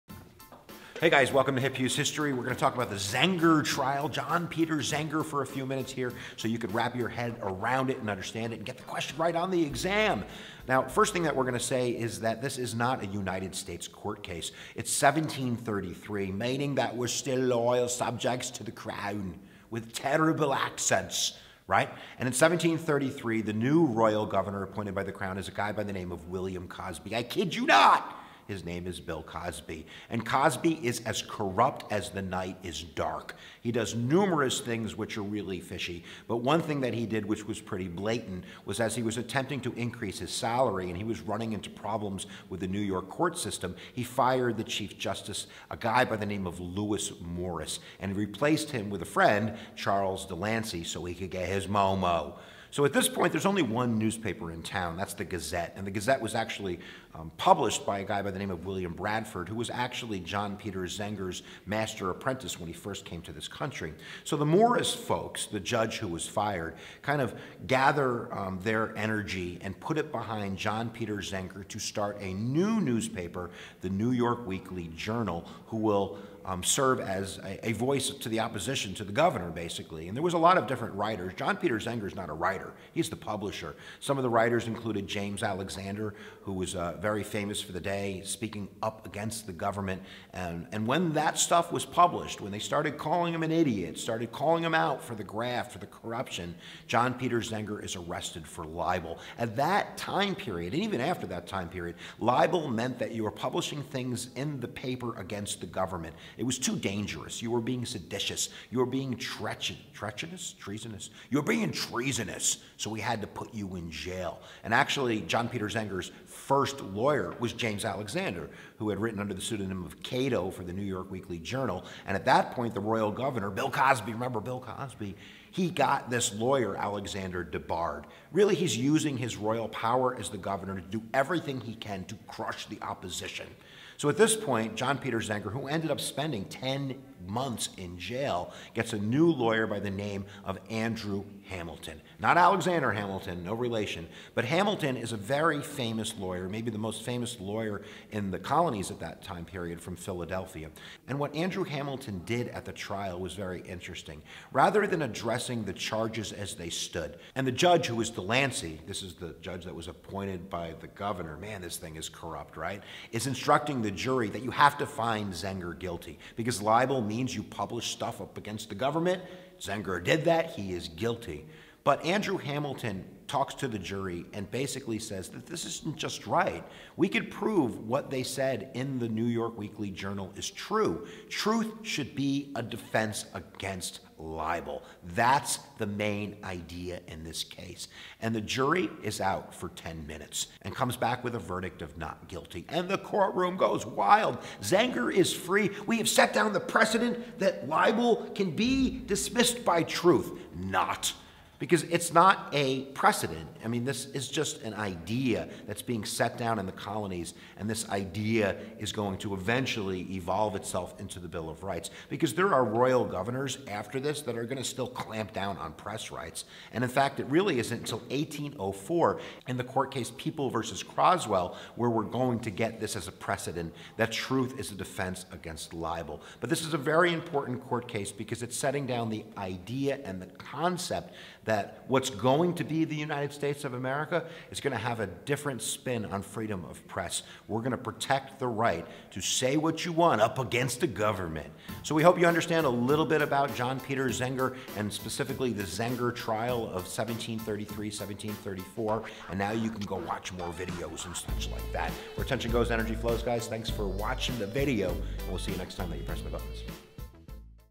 The Zenger Trial Explained
A quick review of the John Peter Zenger Trial of 1734 and it's consequences on the development of Freedom of the Press.